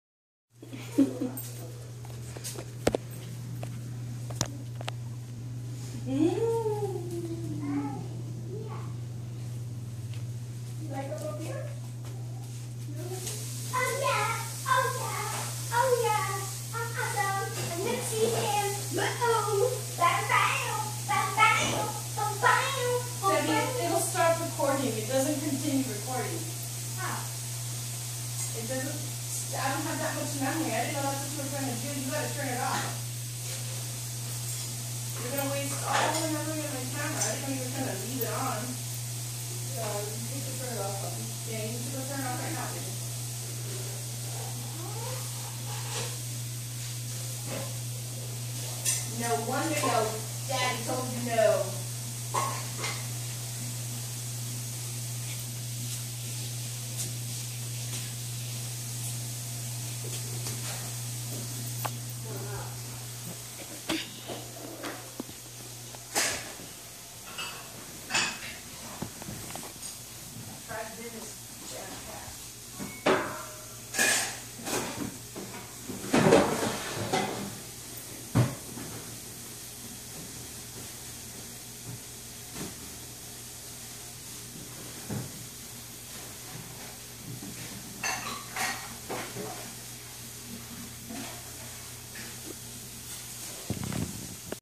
Sebi the Elf Catcher
Seb trying to set up hidden camera in attempt to catch Jingle flying